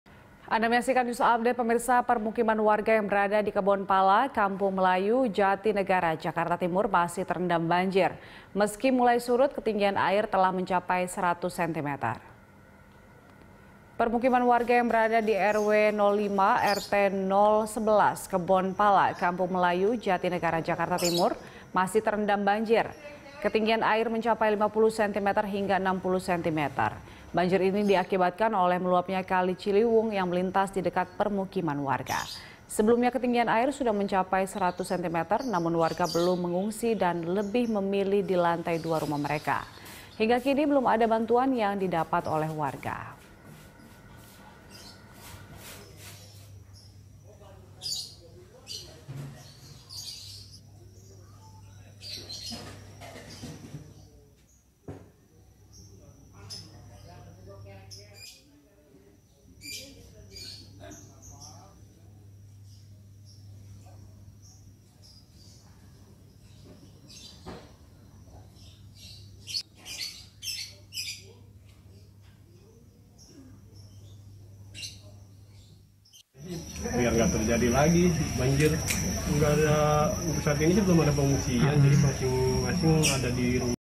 Permukiman Warga di Kampung Melayu Masih Terendam Banjir
Permukiman warga di Kebon Pala, Kampung Melayu, Jatinegara, Jaktim, masih terendam banjir. Meski mulai surut, ketinggian air mencapai 100 centimeter.